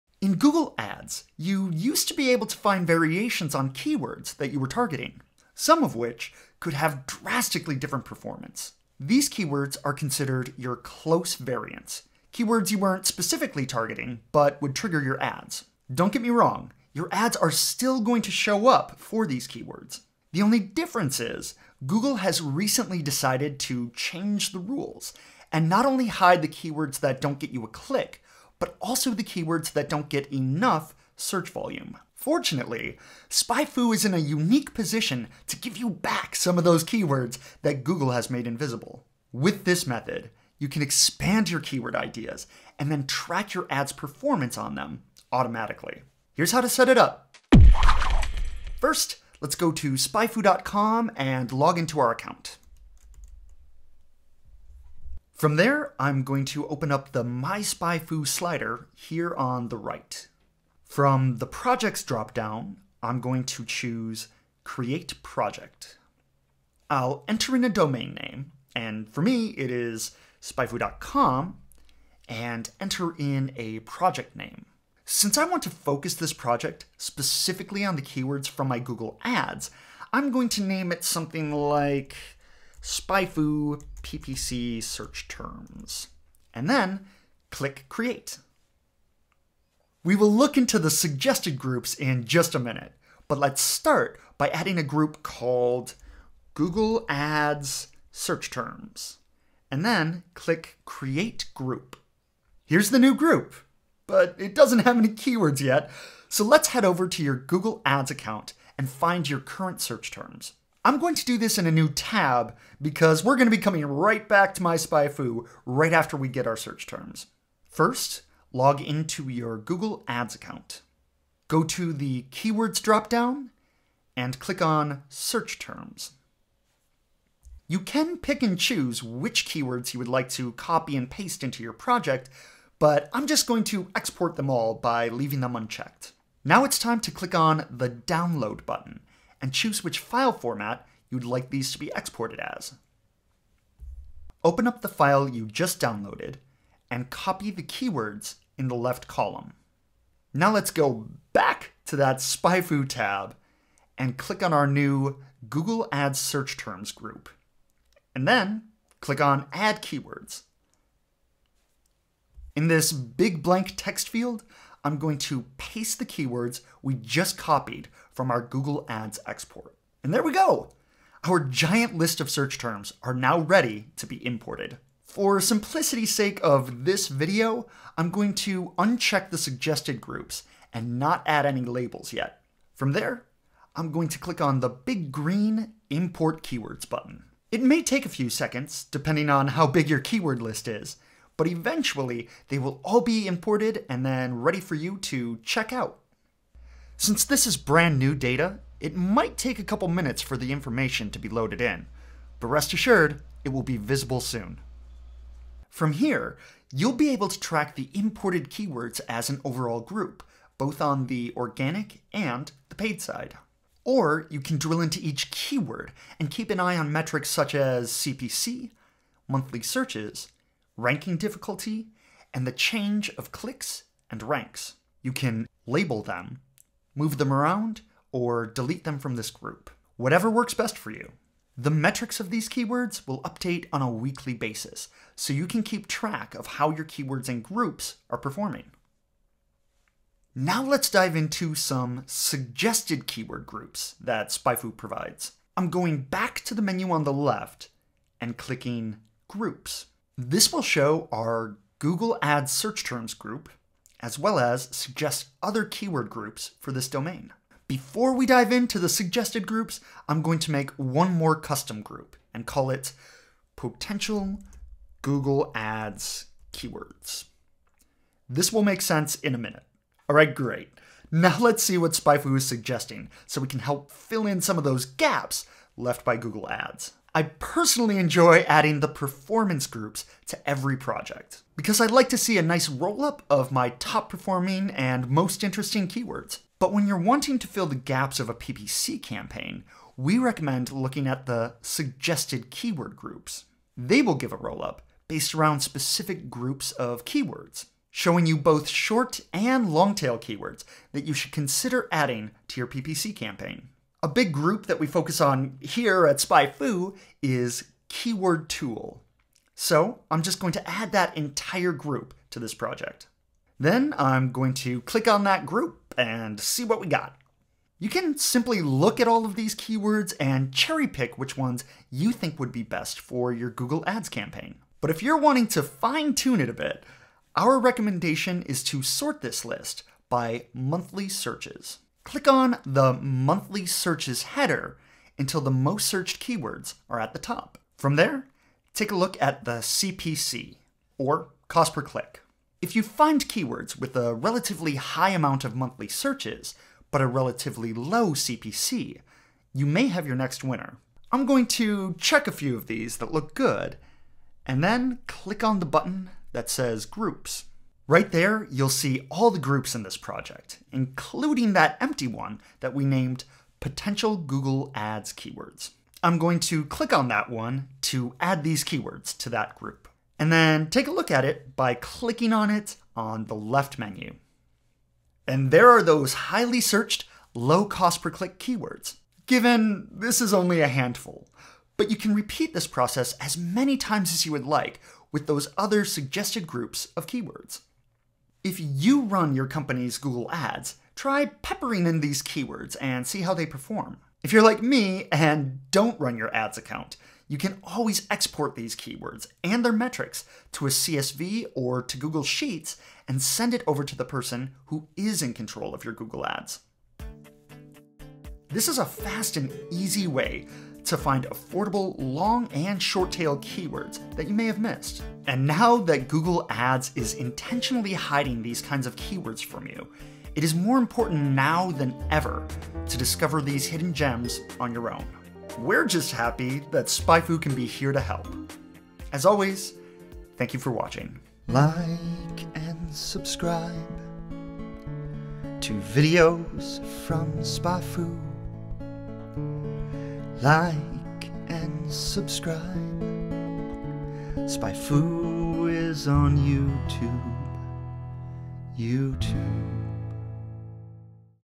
Find More Search Variants with SpyFu
In Google Ads you used to be able to find variations of keywords that you were targeting.

Some of which could have drastically different performance.

These keywords are considered "close variants” - keywords you weren’t specifically targeting, but would trigger your ads.

Google has recently decided to change the rules and hide not only the keywords that didn’t get you a click, but also the ones that don’t get ‘enough’ search volume.

Fortunately, SpyFu is in a unique position to give you back some of those search terms that Google has made invisible.

With this method, you can get expanded keyword ideas and then track your ad's performance on them--automatically.

We will go over:
0. Start (0:00)
1. Setup (0:46)
2. Finding Search Terms in Google Ads (1:42)
3. Adding Search Terms to MySpyFu (2:30)
4. Analyzing Search Terms (3:24)
5. Adding Search Variants (4:17)
6. Getting the Best Search Variants (5:20)
7. Exporting Search Variants (6:27)
8. Wrap-Up (6:48)